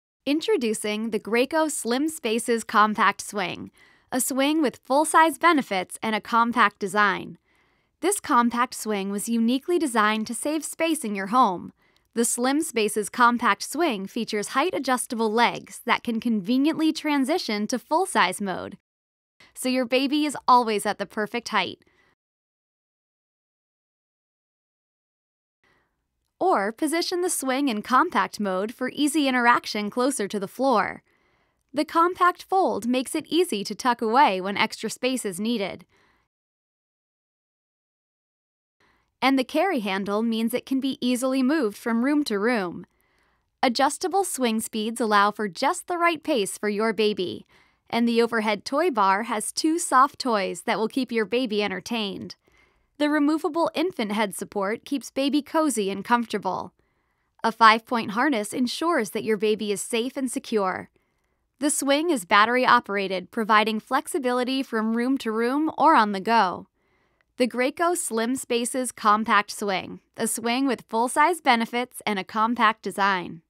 Graco Slim Spaces Compact Swing
Experience the space saving benefits of a compact swing without sacrificing the easy interaction with baby that you find with a full size swing. The Graco® Slim Spaces® Compact Swing features height adjustable legs, allowing mom to choose between compact or full size mode, and ensures baby is always at the perfect height for interaction. The compact fold makes it easy to tuck away when extra space is needed. The carry handle and battery operated motor make this swing ideal for ultimate portability.